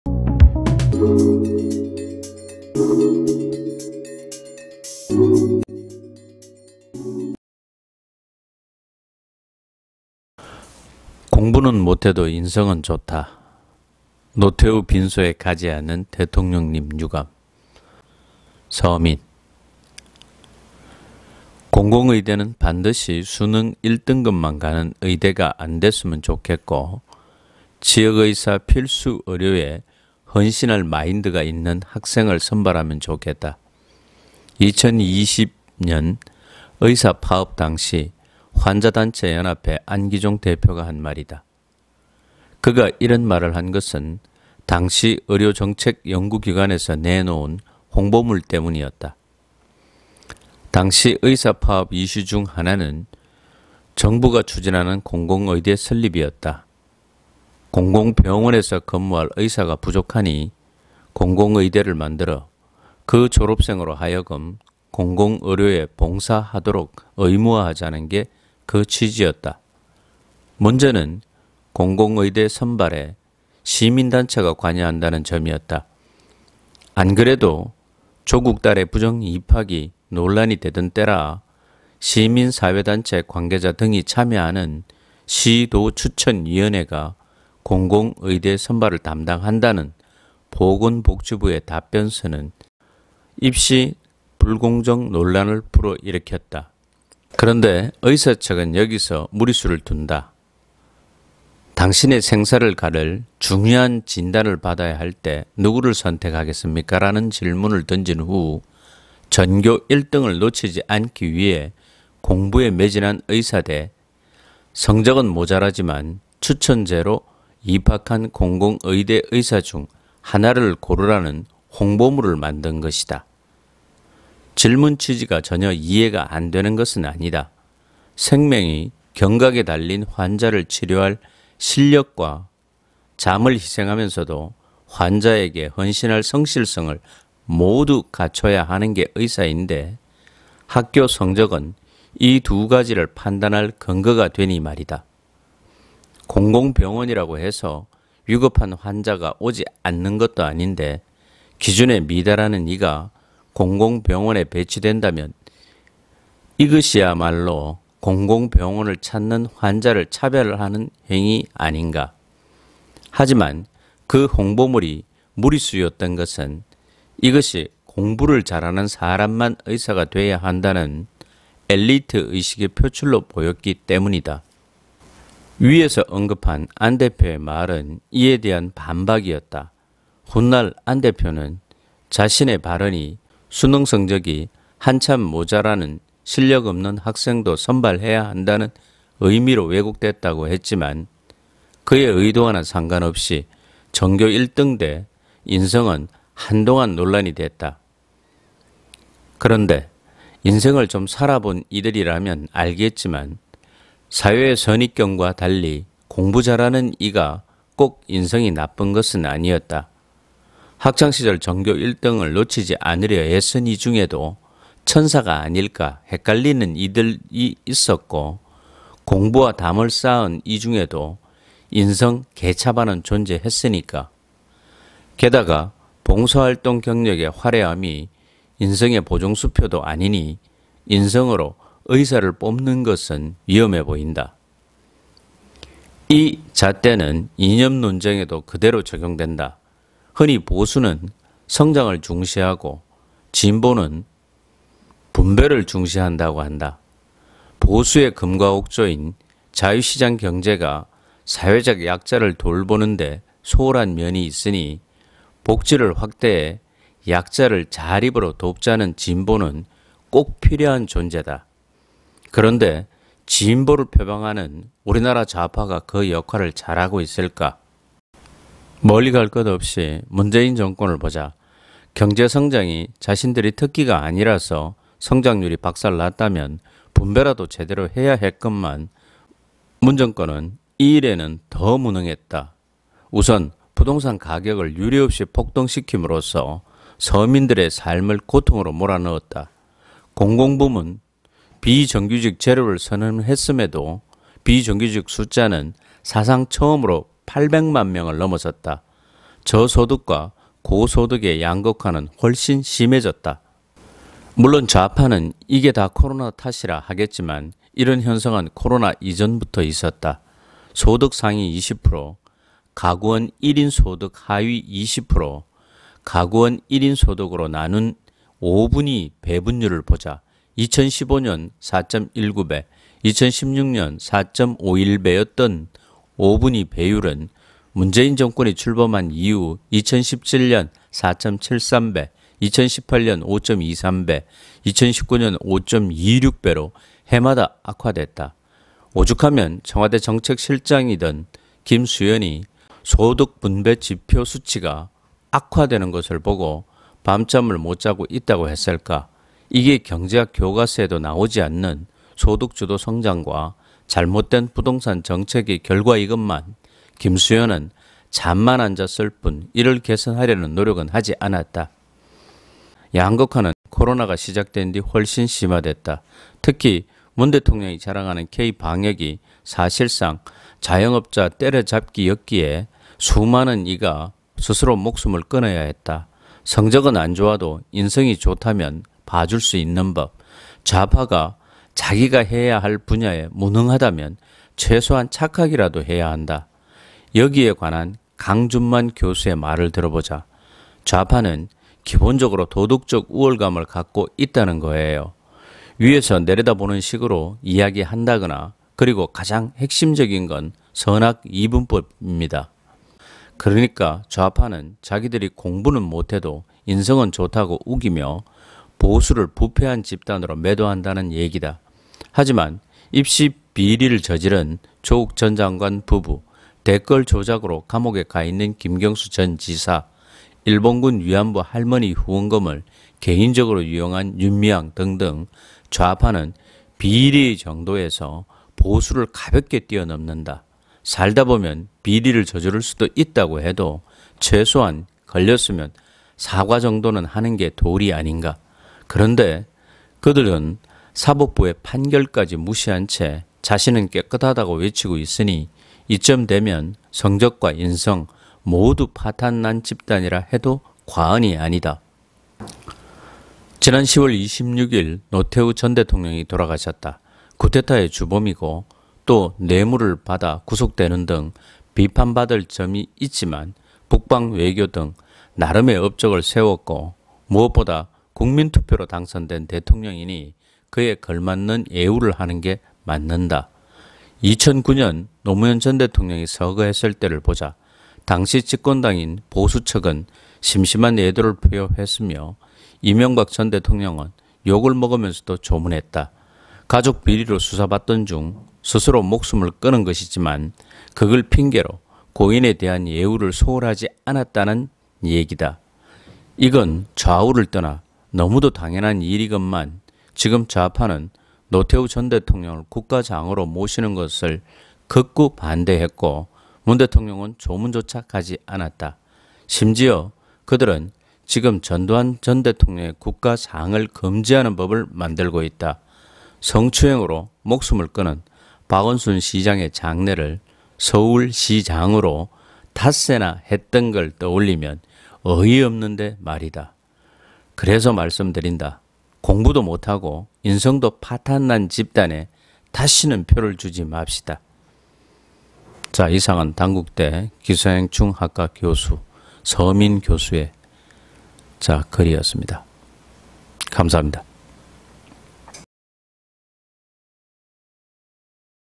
"공부도 못하고 인성도 파탄 난 좌파 다시는 표를 주지 맙시다" 서민
서민 "공부도 못하고 인성도 파탄 난 좌파 다시는 표를 주지 맙시다"잘못을 사과하지않는 위선적인 진보 좌파 재집권은 막아야한다 문파타파